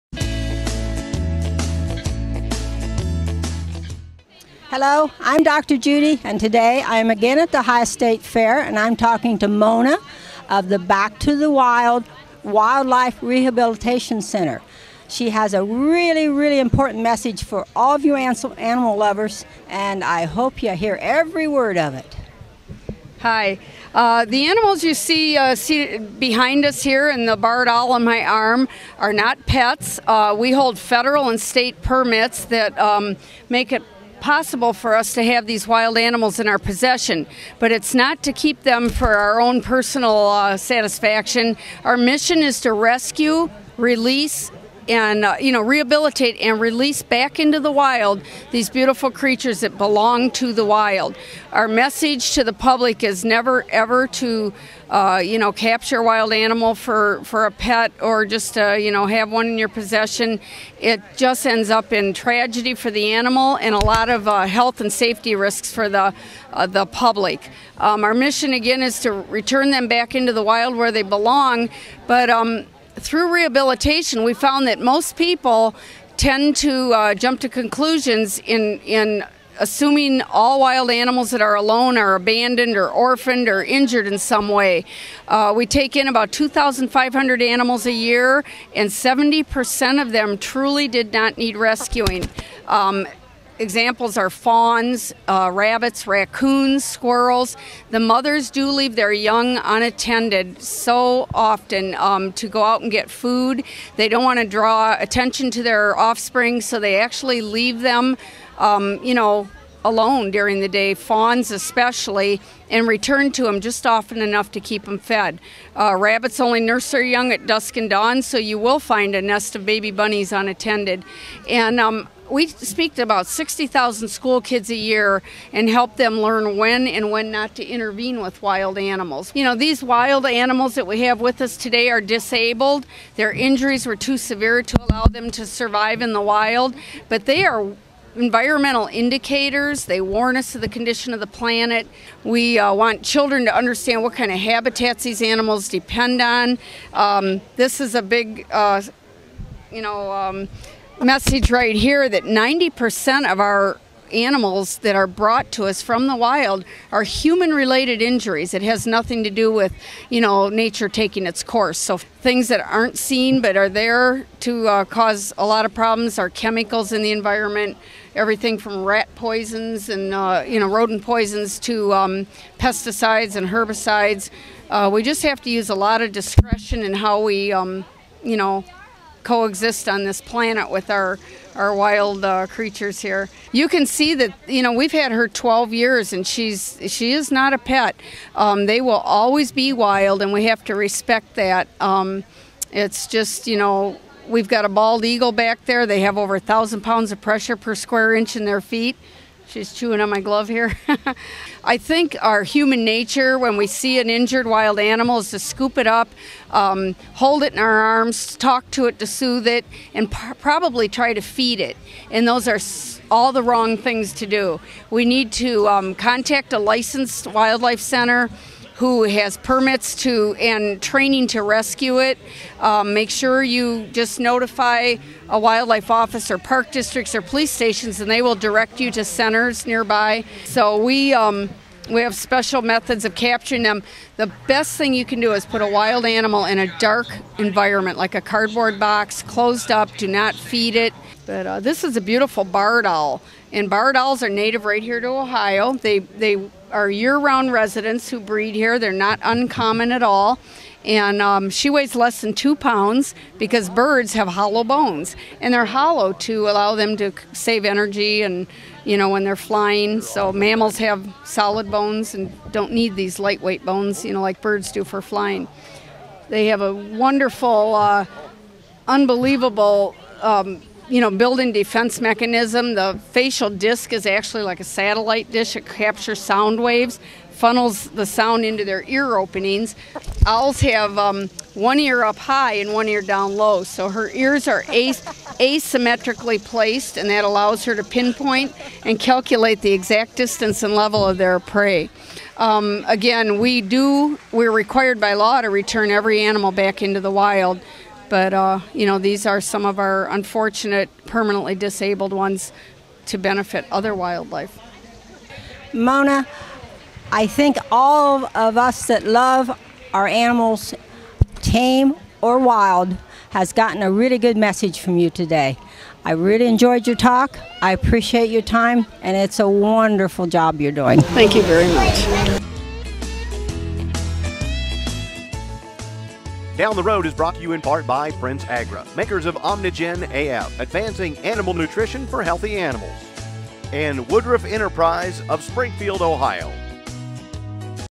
Dr Judy talks about Rehabilating Wild Animals & Birds
Dr. Judy  Sanders talks Mona Rutger of Back to the Wild Rehabilitation Center